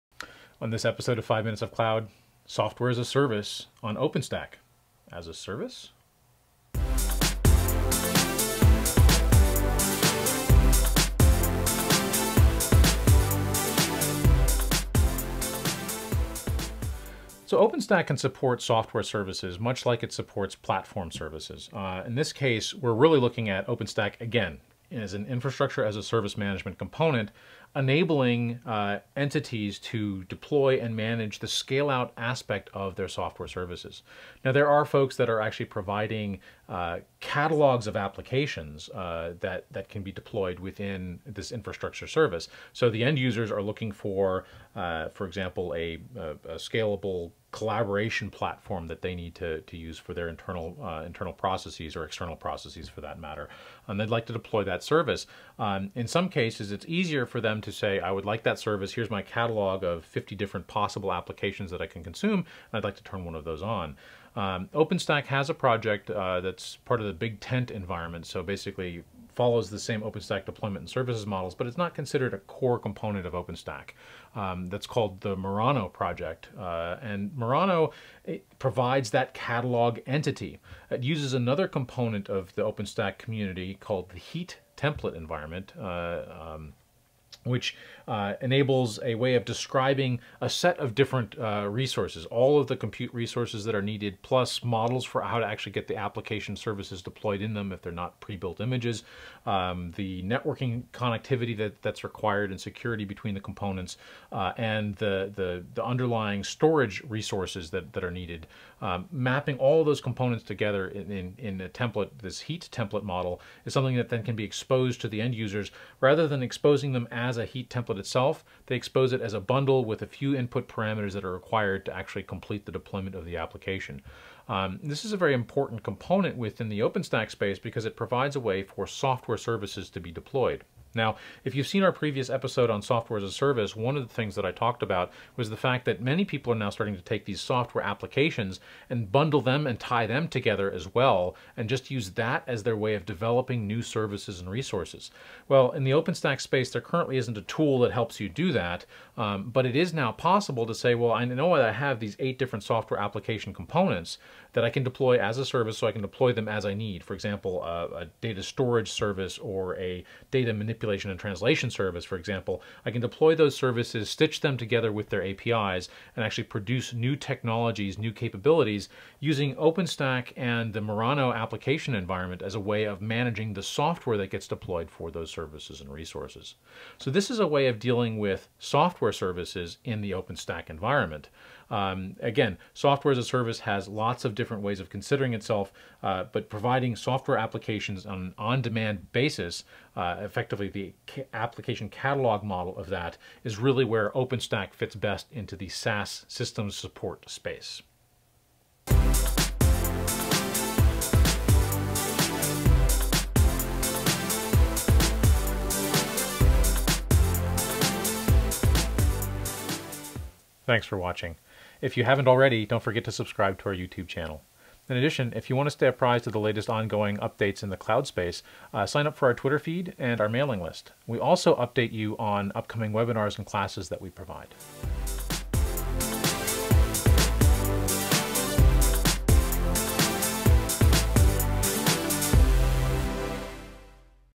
OpenStack for Software-as-a-Service (SaaS) Clouds 5MoC-39
OpenStack is, first and foremost, built to provide an Infrastructure-as-a-Service, however, its versatility has lent it to breaking out of that box and being used to build and support both PaaS and SaaS clouds. In this episode Robert Starmer, the CTO of Kumulus Technologies discusses how SaaS works and how specifically the Murano (Application Catalog) and Heat (Orchestration) projects enable SaaS on top of OpenStack.